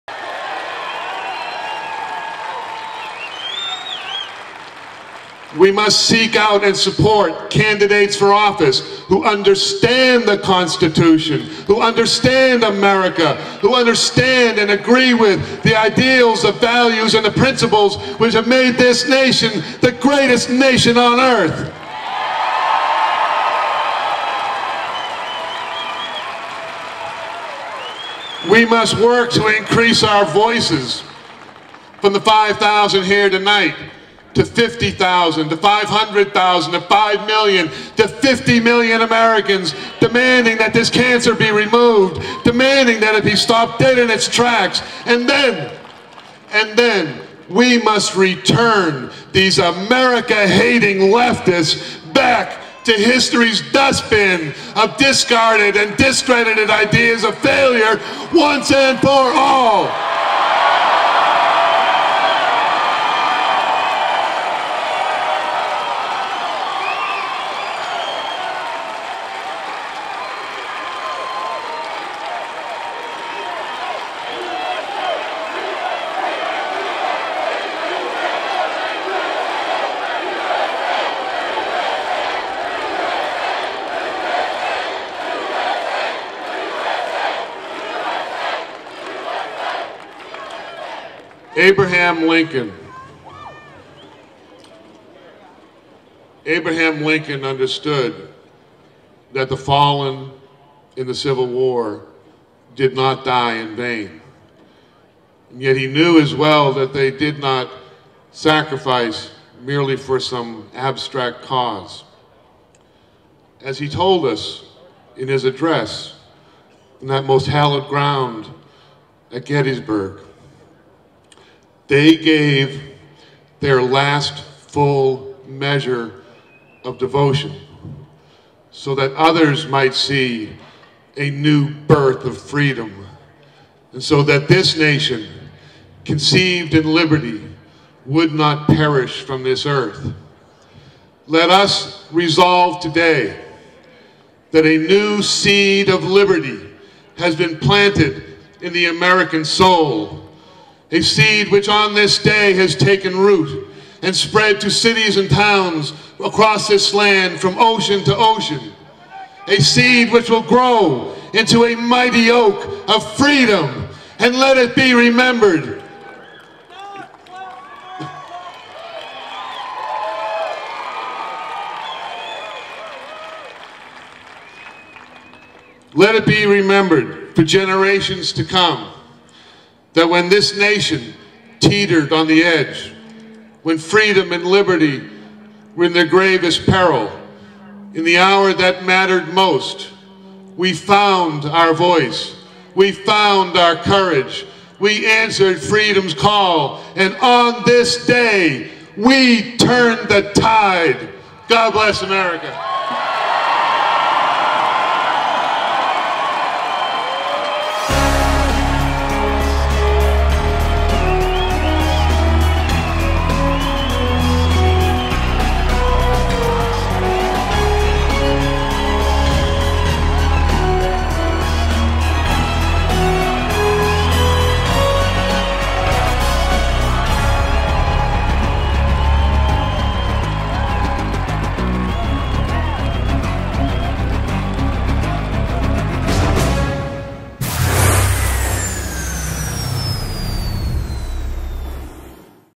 Adrian Murray Official Fort Worth Tea Party Video 3
Adrian Murray speaks at the Fort Worth Tax Day Tea Party on 04.15.09 at La Grave Field on Fort Worth, Texas. Adrian is just an average American Citizen who cares deeply about our country and the direction it is heading in. He articulates the feelings and thoughts of millions of American citizens and their concerns.